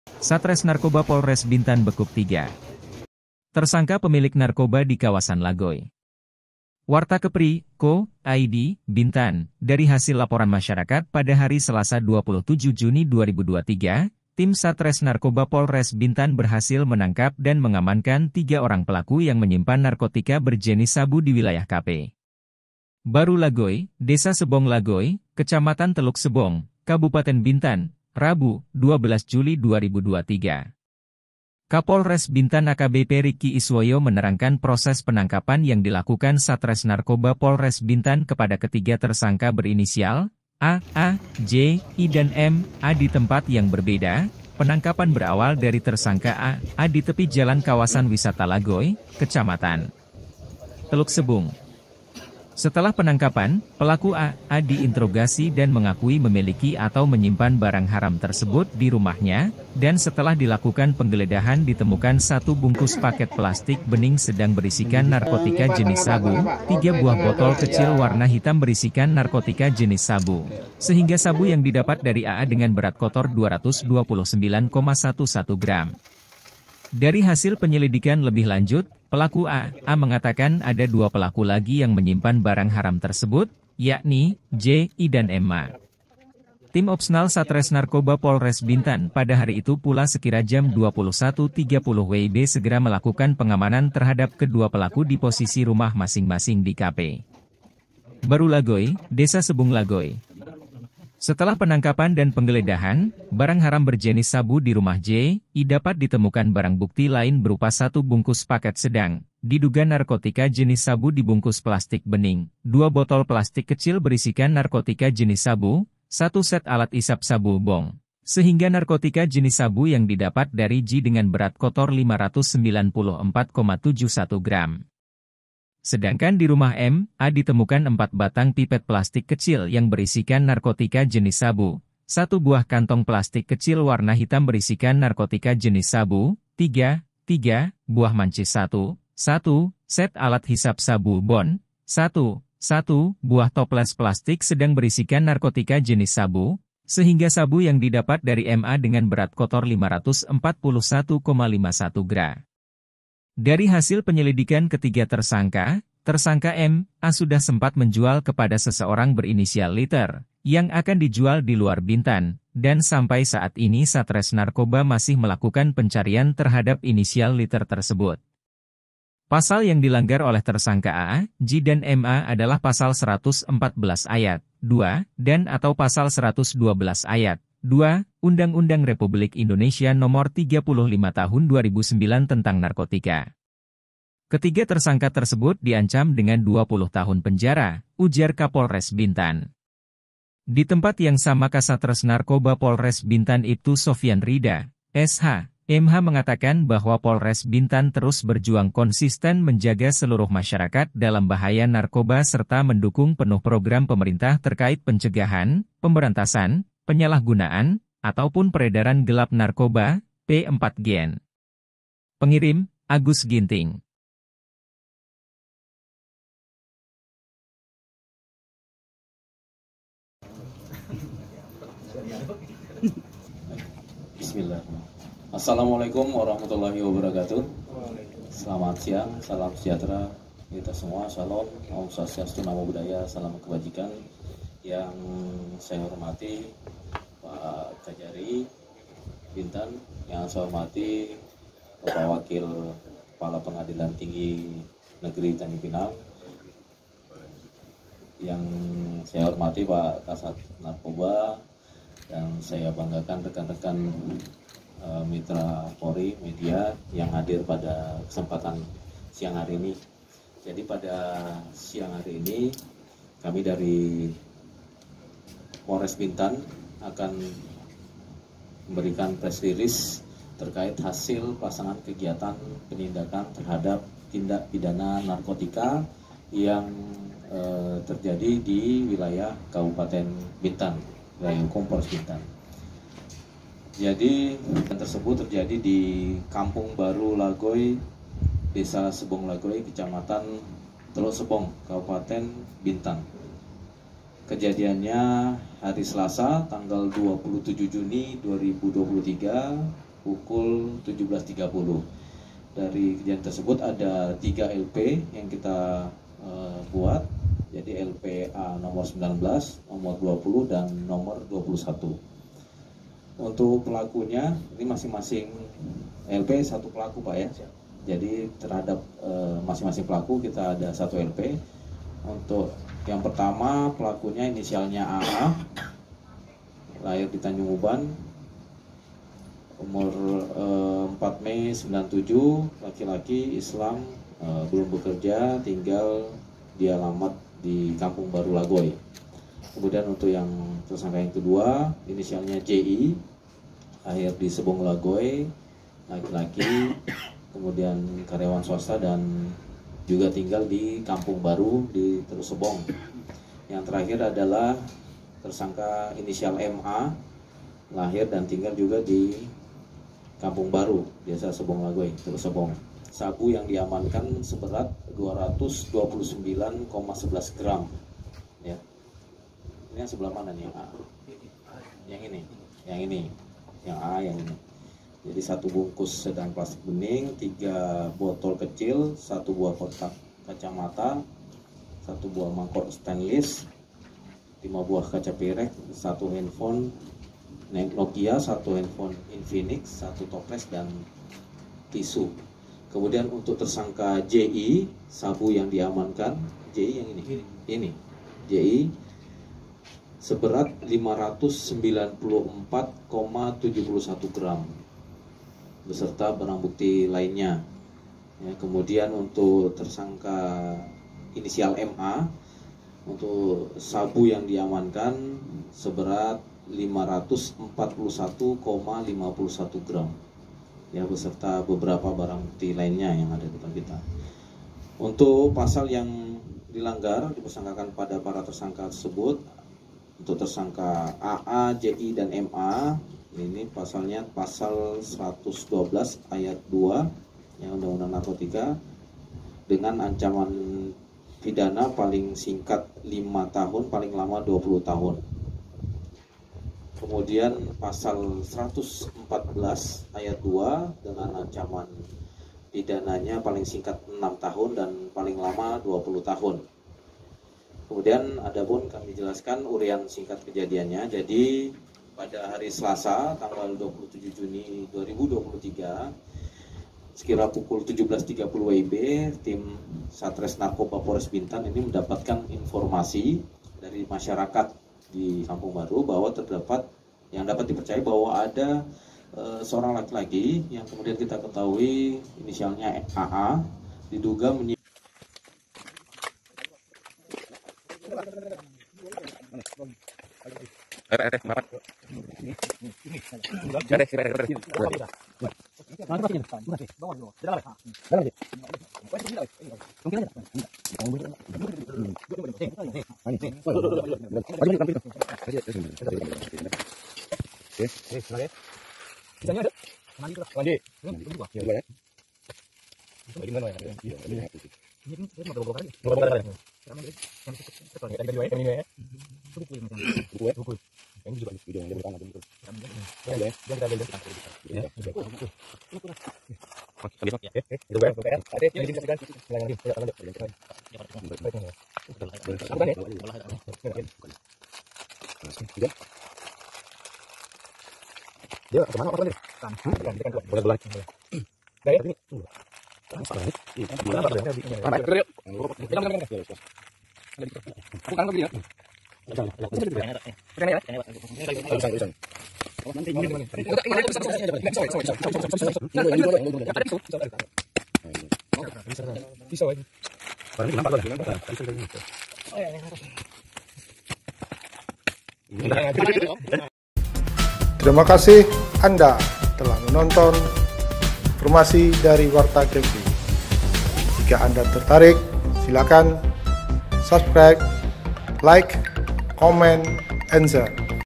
Satresnarkoba Polres Bintan Bekuk 3 Tersangka Pemilik Narkoba di Kawasan Lagoi 2023
wartakepritv Satresnarkoba Polres Bintan Bekuk 3 Tersangka Pemilik Narkoba di Kawasan Lagoi 2023